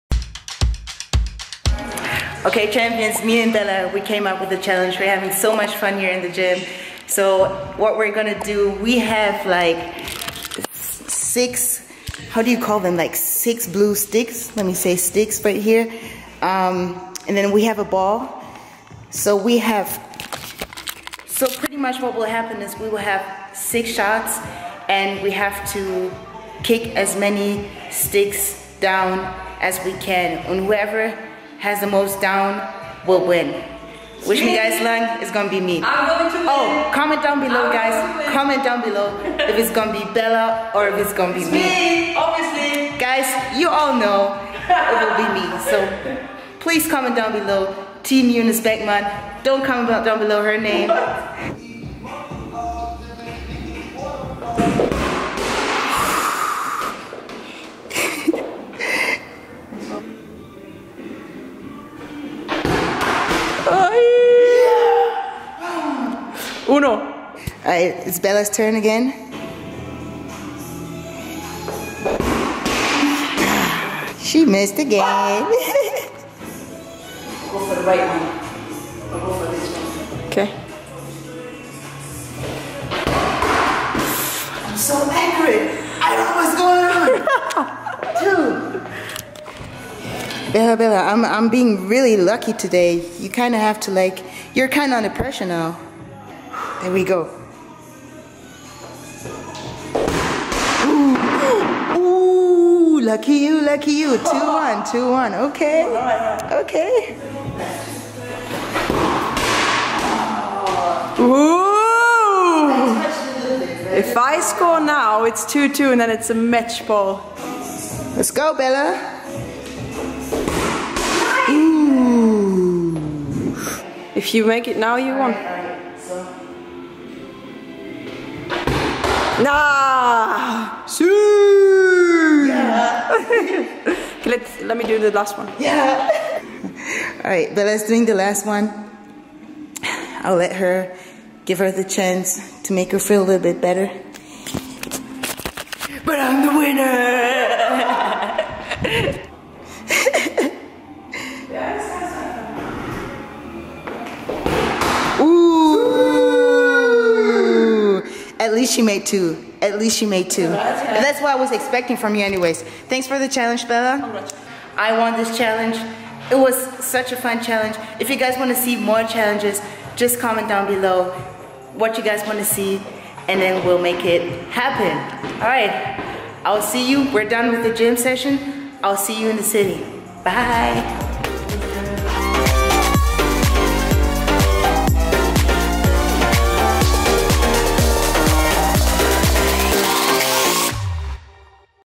Target practice with Eunice Beckmann & Bella Linden
Hey, It's Euniceeeee 🤩
YESSSSS, I BEAT BELLA! 3-1 in the gym sticks challenge! I hope you all enjoyed this weeks challenge! Have you tried the gym tricks challenge yet? Give it a go and remember to tag us using WhySoSerious. Bye Champions!